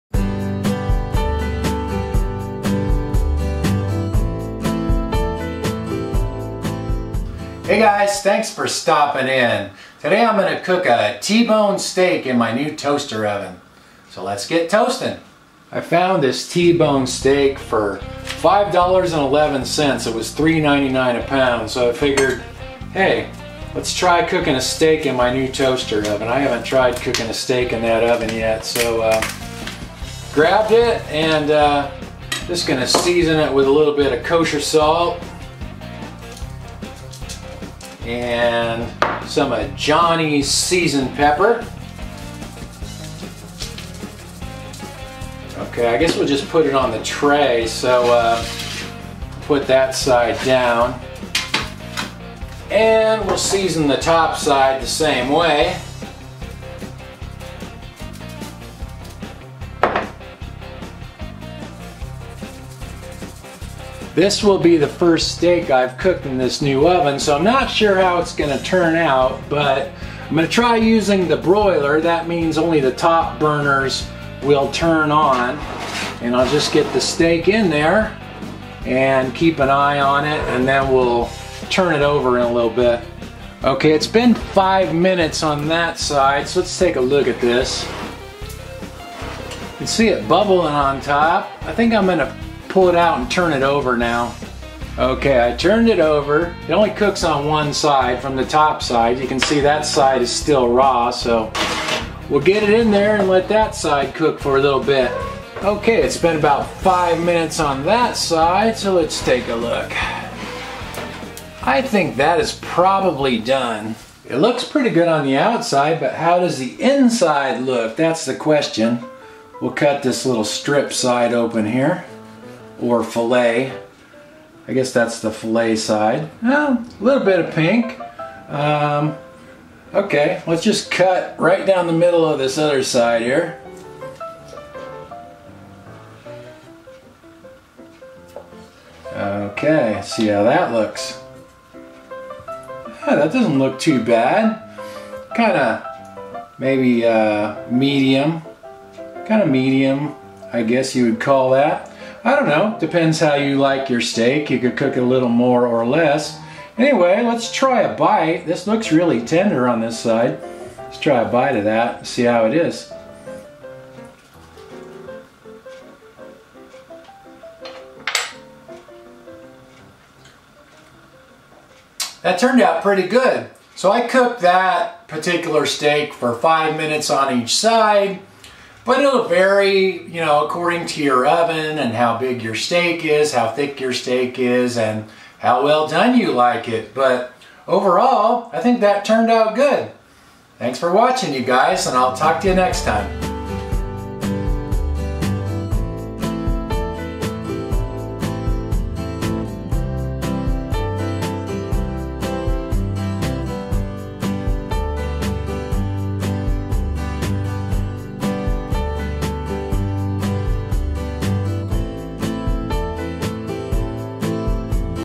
Toaster Oven T-Bone Steak
In this easy cooking video, I cook a T-bone steak in my toaster oven. I used the broil setting and cooked the steak for 5 minutes on each side.